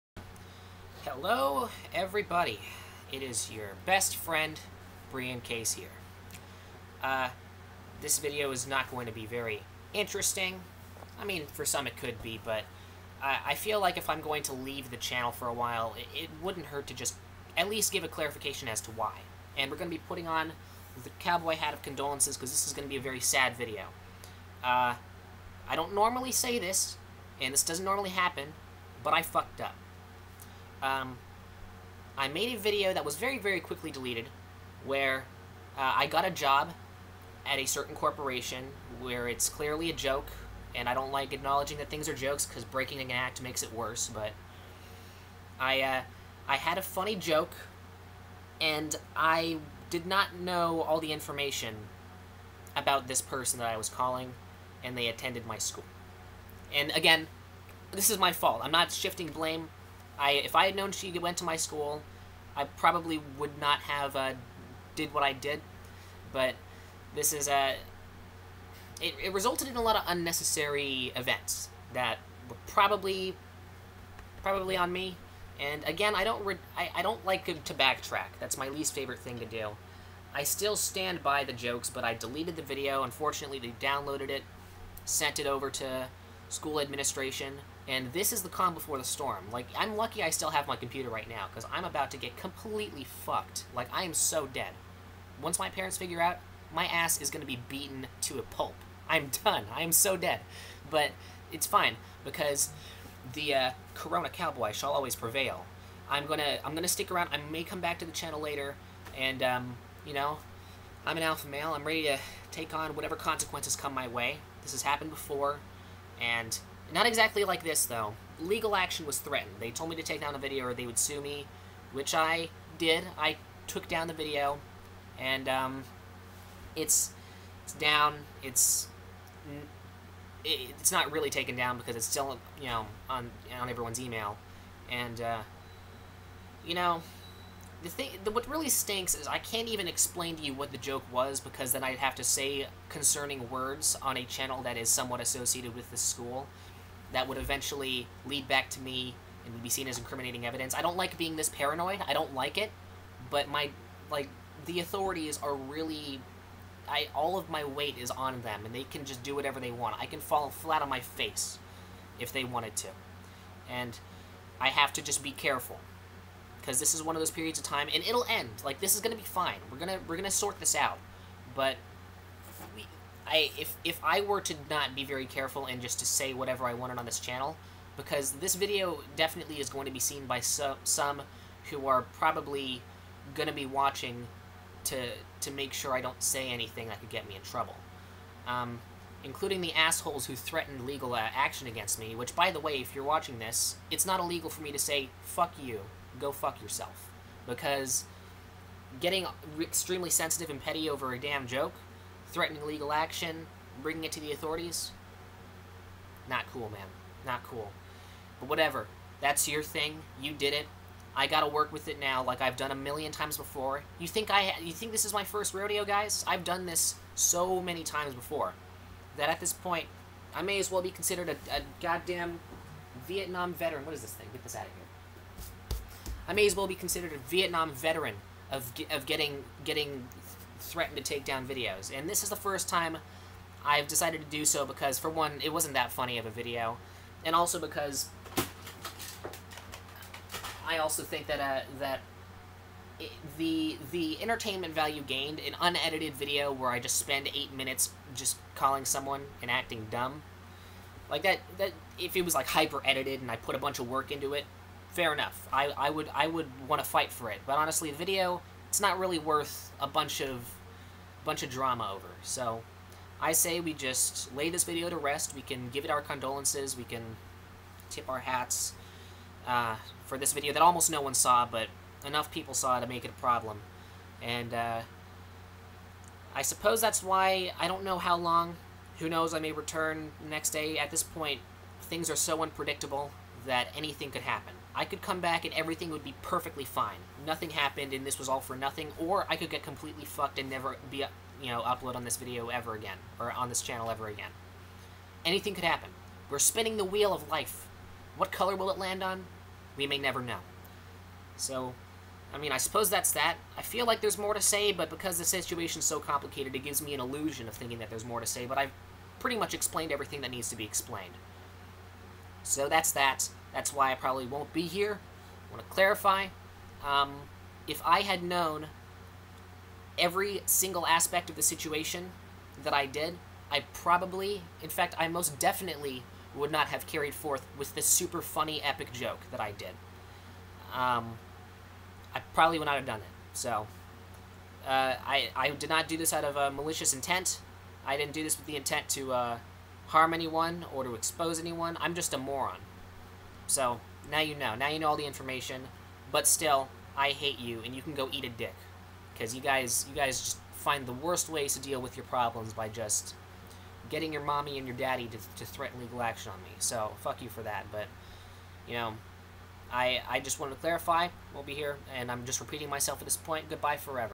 I May Be Gone For A While (additional context in desc.)
this is a truly tragic story. but no seriously i did not mean to be malicious, and while i still stand by my jokes, i did not mean to hurt anyone. it was kinda dumb, i know. prank calling a girl who i didn't know was in my school had pretty clear collateral damage that would come back to bite me in my big, dumb butt wasn't the best move ever. if i had known she would get so scared and upset by my call, i wouldn't have made it. i'm just a moron. i hate backtracking and apologizing, and i still think that, under different circumstances, this would have been fine. however, because people dislike edgy jokes, i am gonna get my ass pounded so hard. don't worry, though. GOOD content shall always prevail.

Also, what I did was not illegal. Threatening legal action is a pretty stinky move on your part.

Also, I have five tumors on my ass.